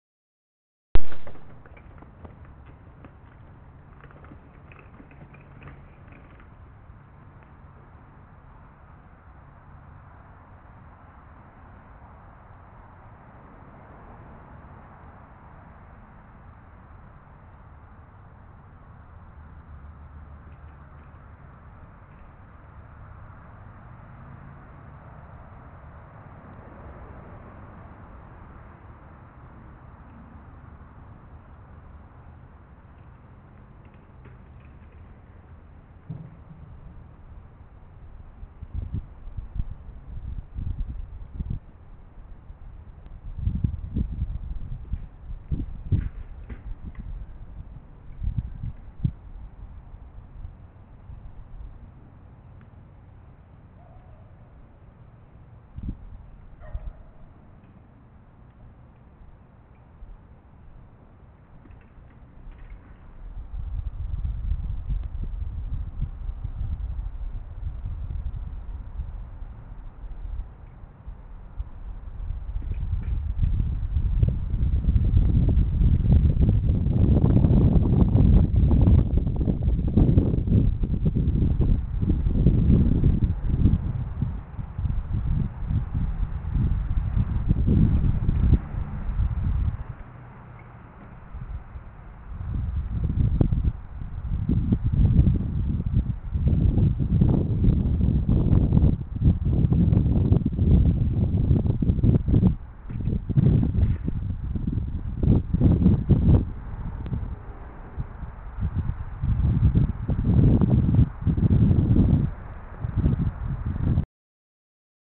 me doing wheelies
me doing crappy wheelies i need a better bike